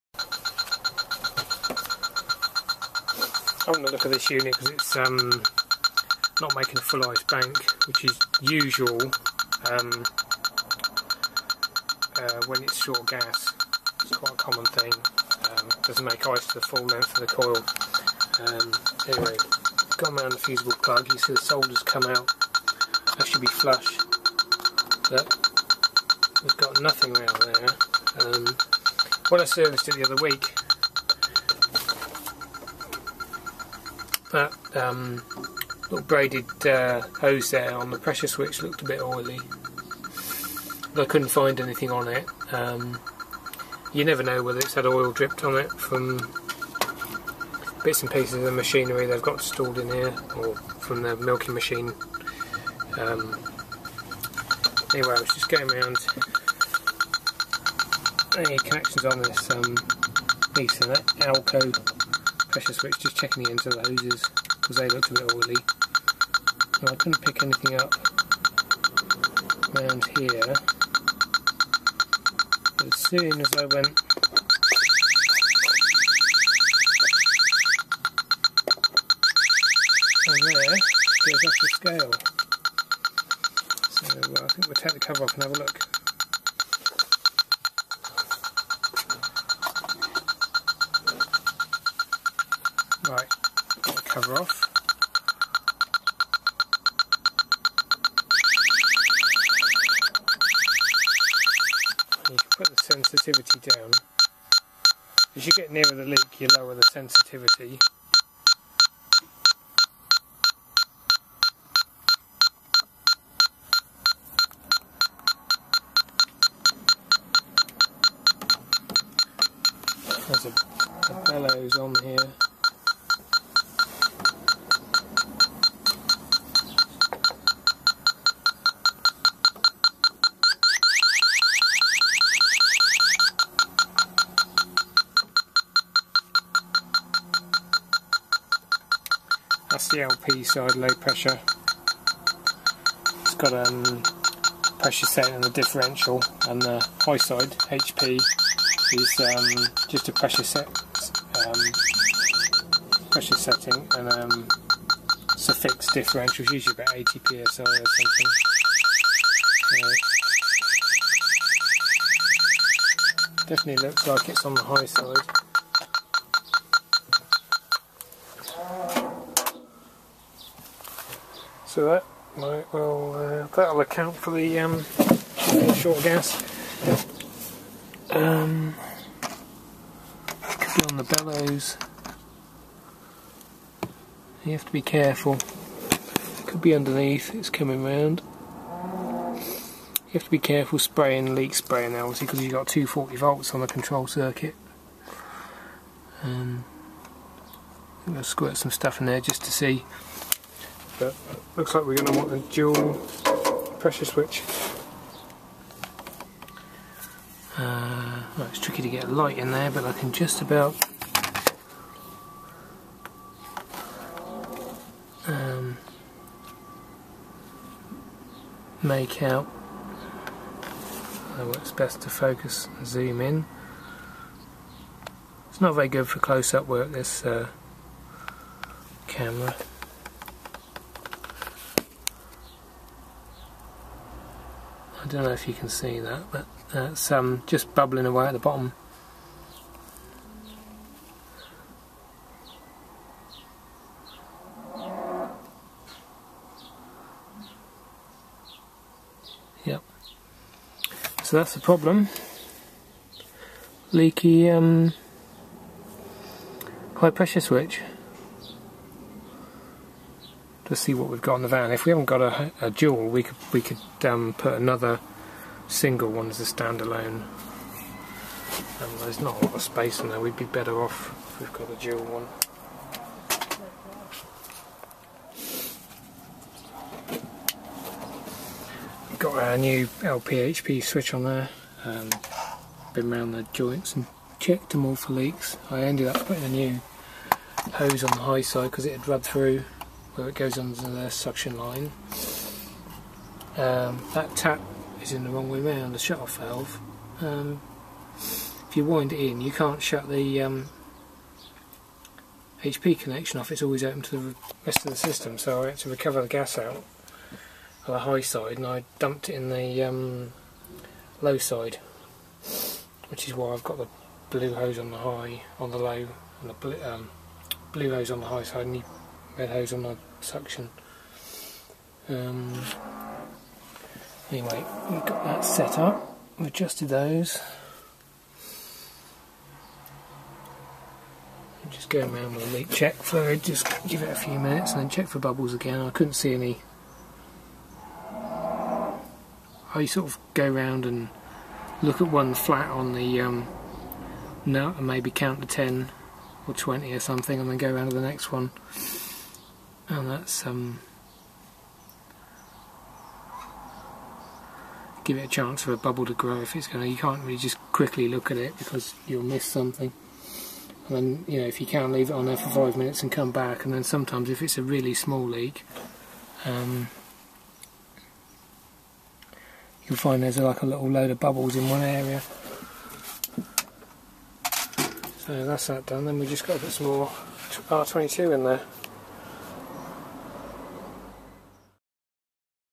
Leaky Low Pressure Switch
Customer reported uneven ice in the iceuiloder which was caused by a shortage of refrigerant.
Checked over the system and found the bellows leaking on the high pressure side of the dual pressure switch. Had a bit of trouble changing it as the shut off valve in the discharge pipe was installed backwards at the factory so i had to recover the charge from the high side.